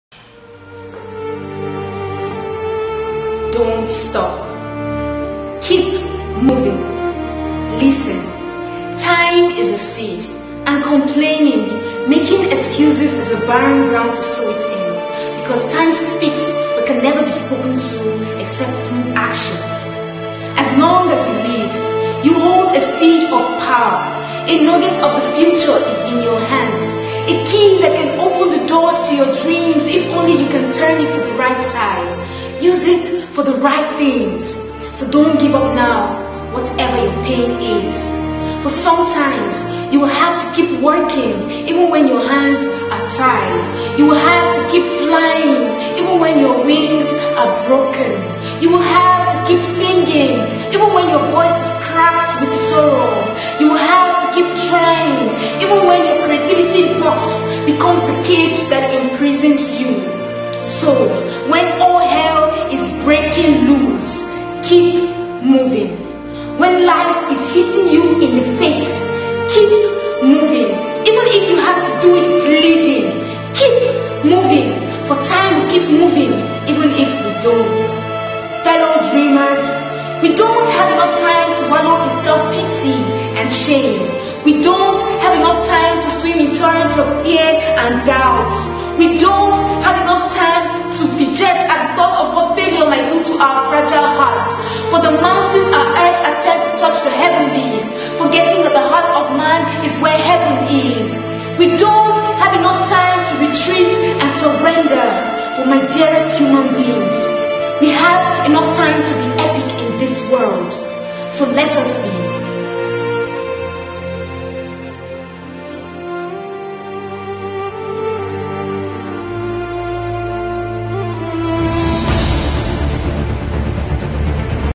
KEEP MOVING | SPOKEN WORD POETRY
If you're  going through a hard time right now, this is for you. I put this poem together to encourage you to persevere if you feel like giving up.